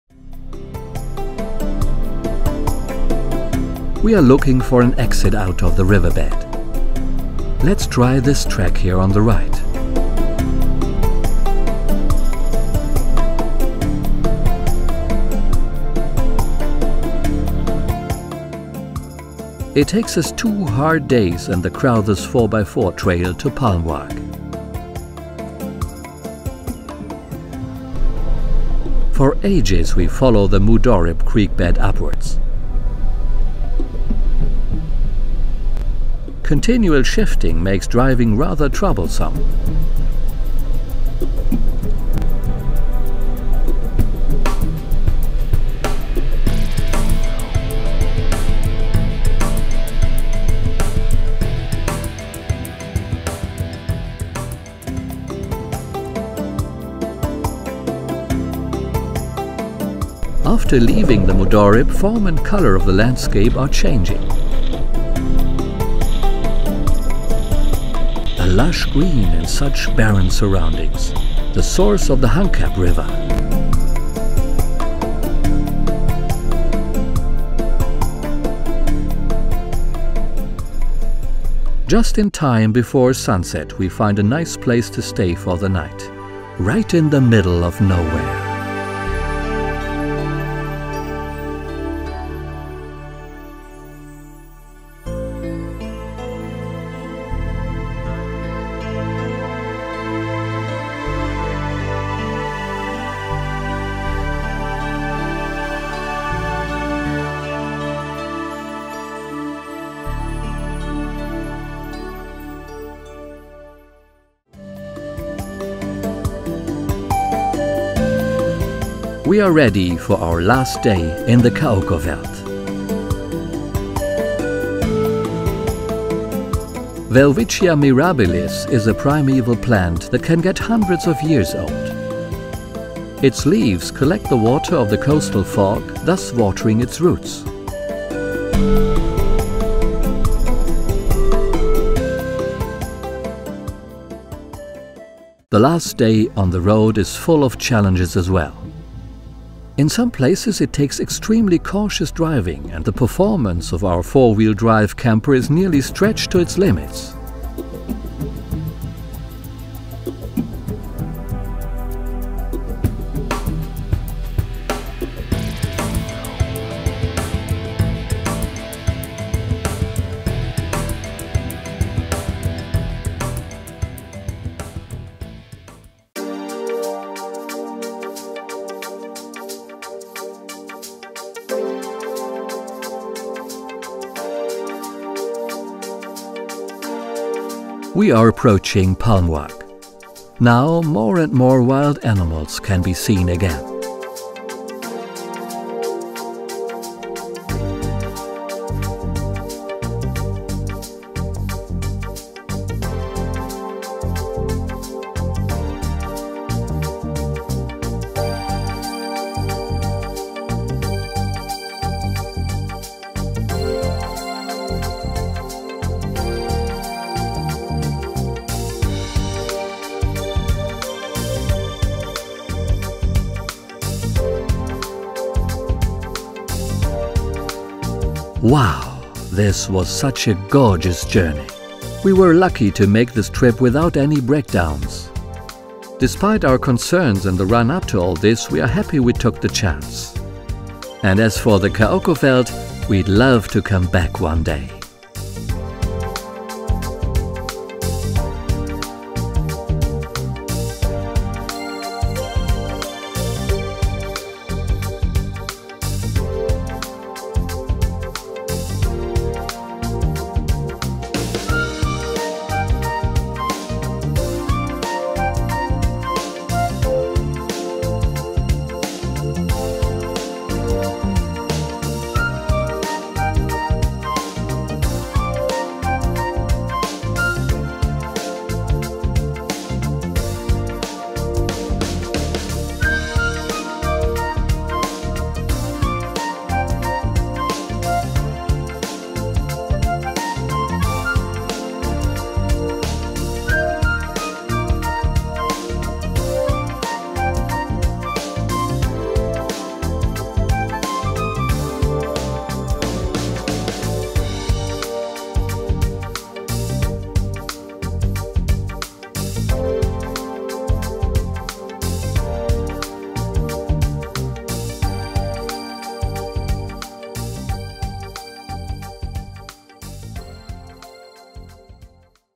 Kaokoveld Part 5 - English
BeschreibungNot knowing, if the Mercedes Sprinter Camperwill make it, we entered the Kaokoveld through the D3707. The start of a wonderful and adventures trip. The night Sky, the small villages, the desert Elephants, the Himba and the riverbeds are some of the highlights. More then just a Road-Movie!